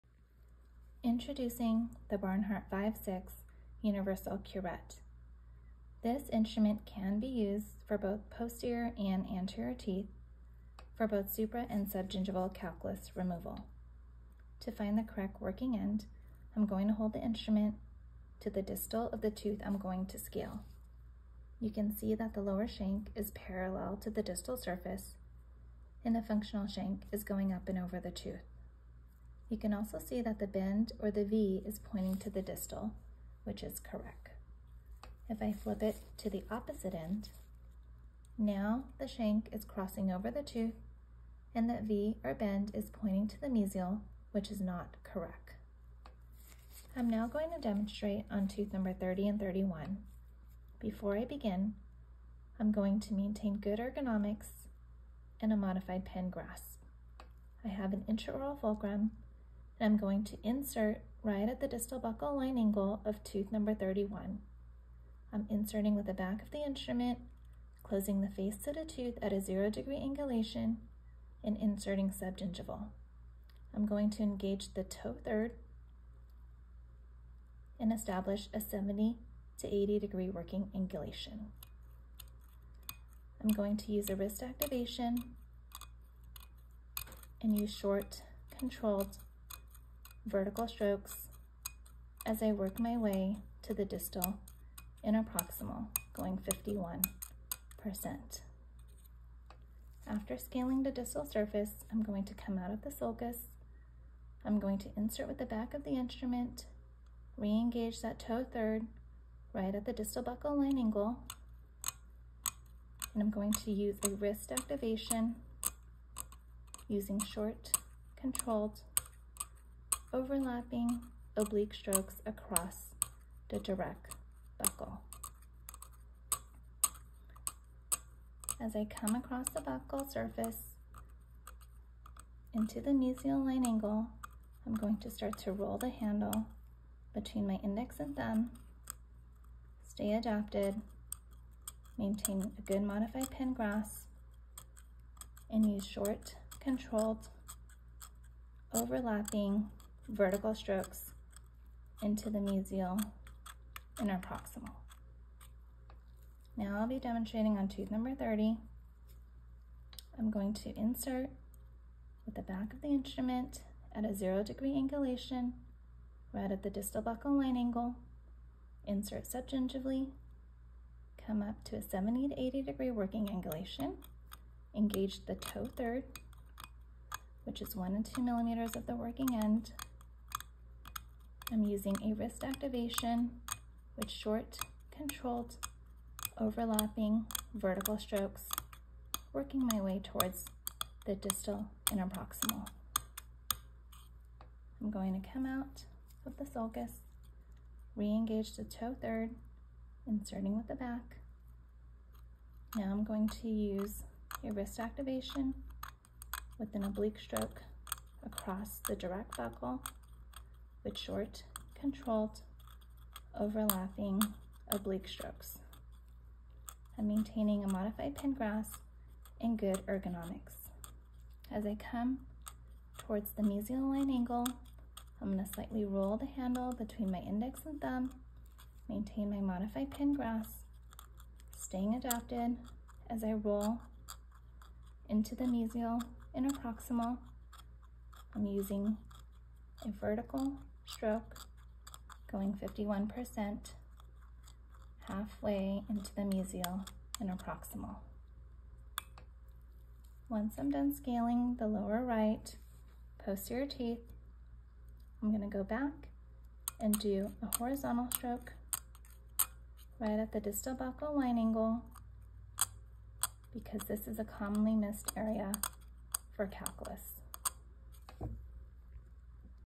Universal Curette Barnhart 5/6- Posterior Teeth
Demonstration of the Universal Curette Barnhart 5/6 on teeth numbers 30 and 31.

This instrument is used on posterior and anterior teeth for supra and subgingival calculus removal.